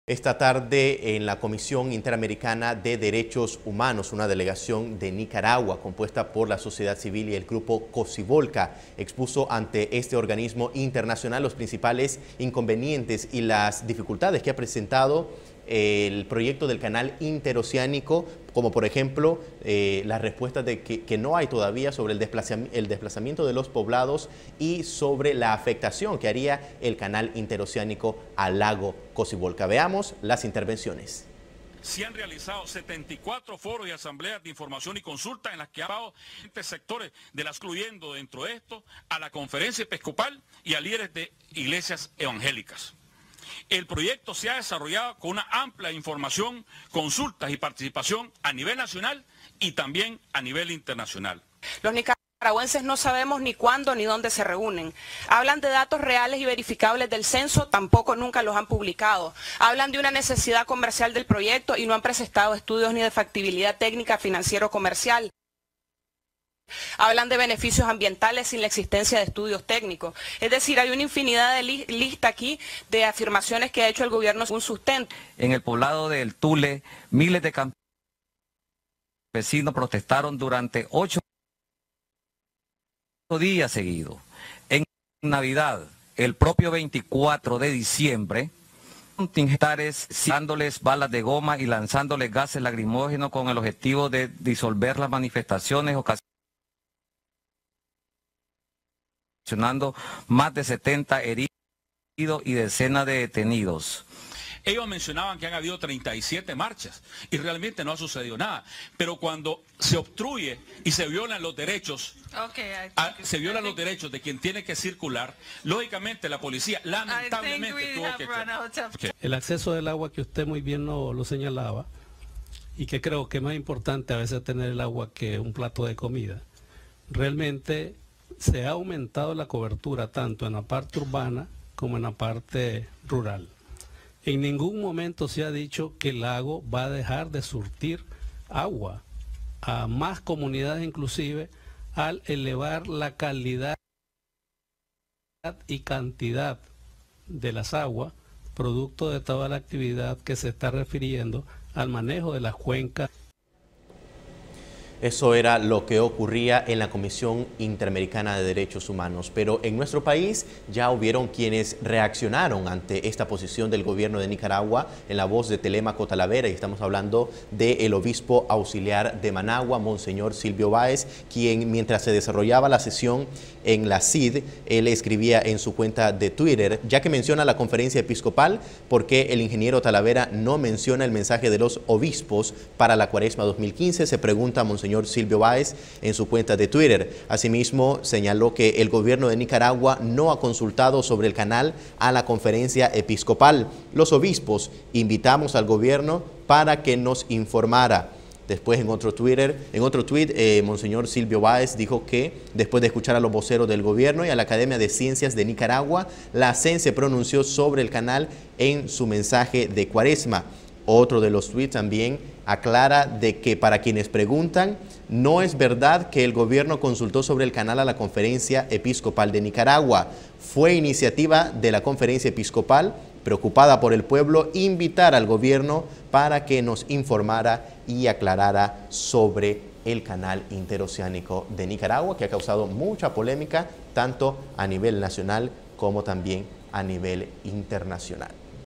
Audiencia en la CIDH sobre el canal interoceánico de NIcaragua
Estado nicaragüense fue sentado en el banquillo de la Comisión Interamericana de los Derechos Humanos para responder sobre violaciones a los DDHH de los nicaragüenses con la construcción del canal.